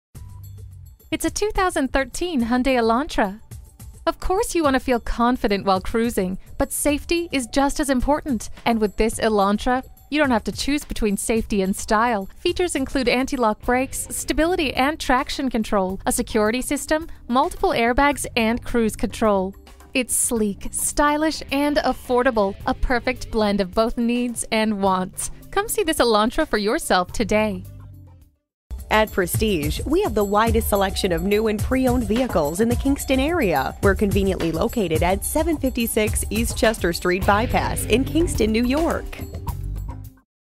2013 Hyundai Elantra Kingston NY Poughkeepsie, NY DH357302 - SOLD
Year: 2013
Make: Hyundai
Model: Elantra
Trim: GLS
Engine: 1.8L 4-Cylinder DOHC 16V Dual CVVT
Transmission: 6-Speed Automatic with Overdrive
Color: Silver
Mileage: 36350
Stock #: DH357302
VIN: 5NPDH4AE6DH357302
brbrCome take a look at the deal we have on this beautiful 2013 Hyundai Elantra. Hyundai Certified Pre-Owned means you not only get the reassurance of up to a 10yr/100,000 mile limited powertrain warranty, but also a 150-point inspection/reconditioning, 24/7 roadside assistance, trip-interruption services, rental car benefits, and a complete CARFAX vehicle history report. It is nicely equipped Hyundai Certified, 4D Sedan, 6-Speed Automatic with Overdrive, FWD, ABS brakes, ABS w/Electronic Brake Force Distribution (EBD), Dual Heated Power Outside Mirrors, Electronic Stability Control, Heated door mirrors, Illuminated entry, Low tire pressure warning, Remote keyless entry, Remote Keyless Entry System w/Alarm, Tire Pressure Monitoring System, Traction control, and Traction Control System.

Address:
756 East Chester St By-Pass